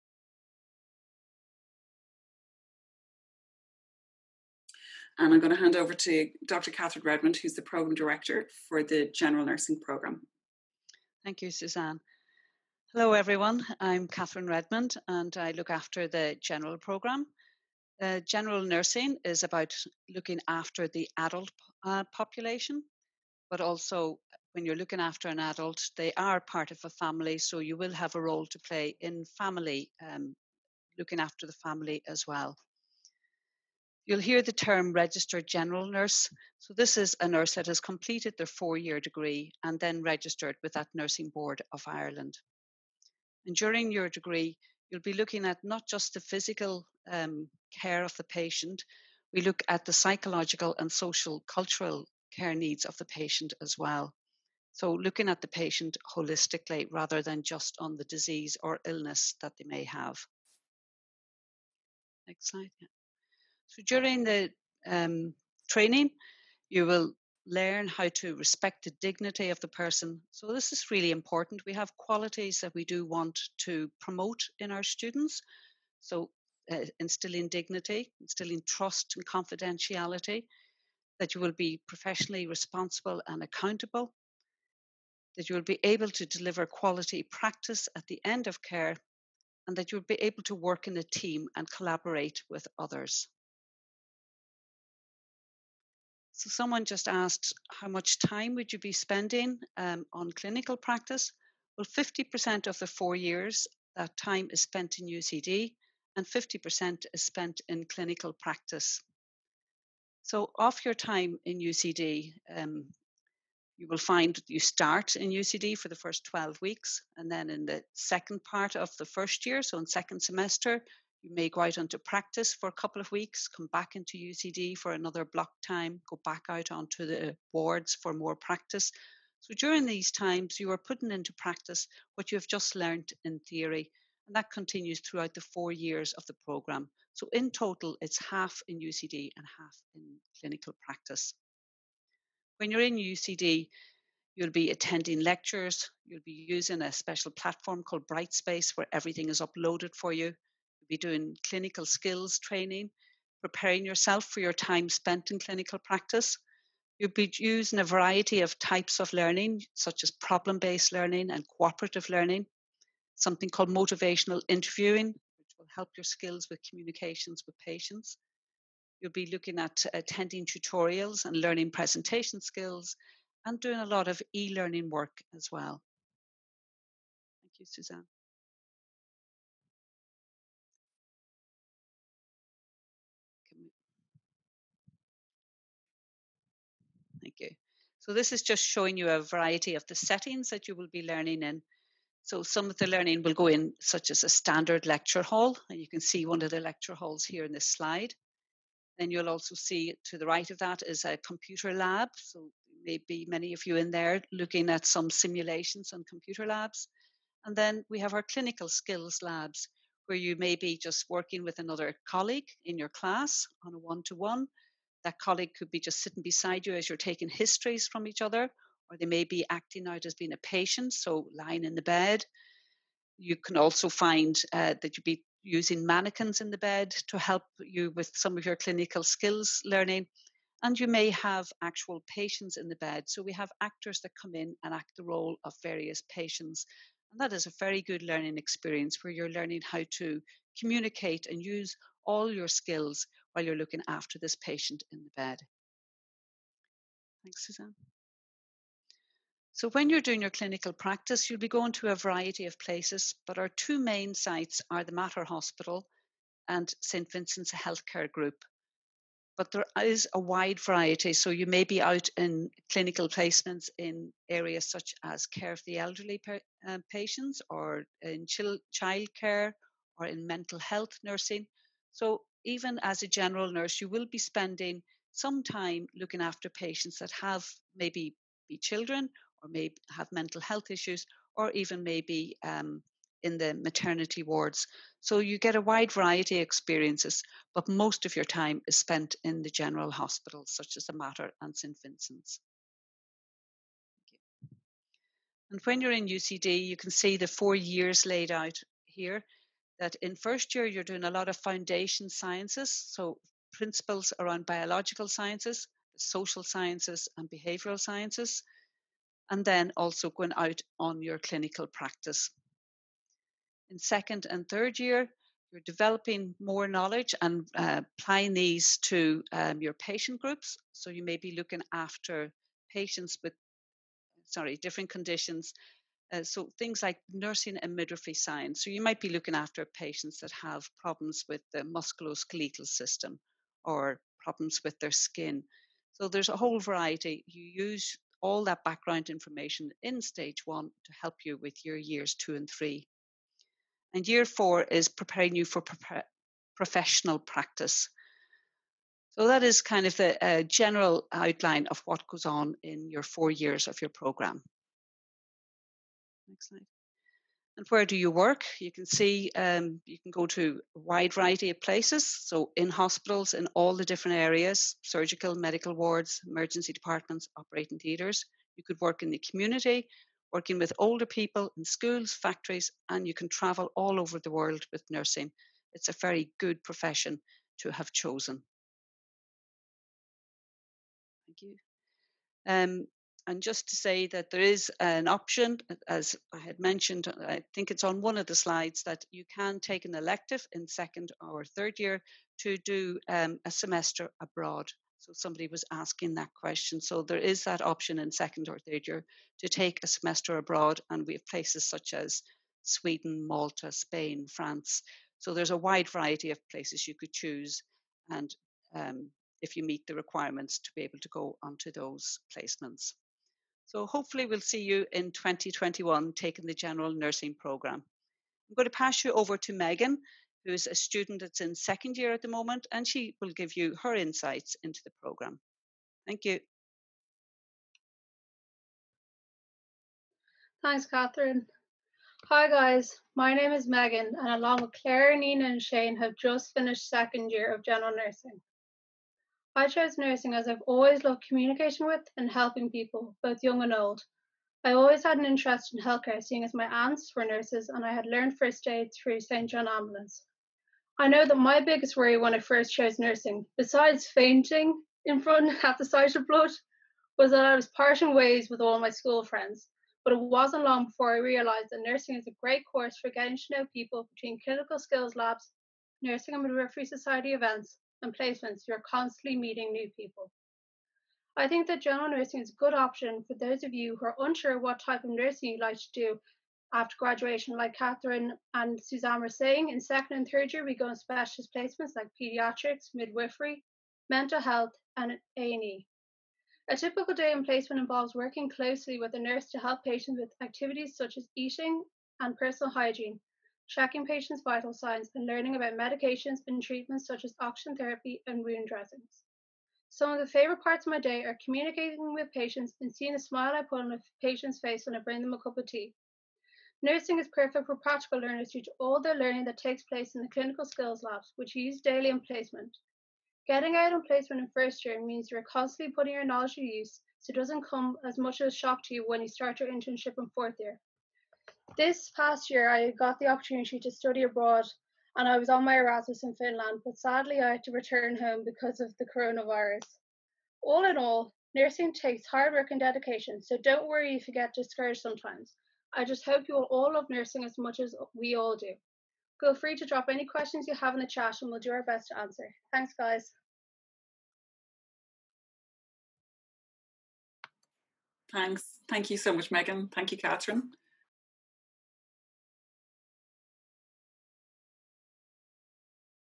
UCD Nursing & Midwifery Virtual Summer School (General Nursing Programme)
Recorded: 3rd June 2020 as part of the UCD Nursing & Midwifery Virtual Summer School webinar

The role of the general nurse is to foster the health and wellbeing of persons experiencing a transition in their health status or  a change in life processes, taking into account their physical, psychological, emotional, social and spiritual needs. Approaches to care include health promotion, disease prevention, disease management, rehabilitation and end of life palliative care. The role of the nurse extends beyond clinical practice to encompass education, research and management.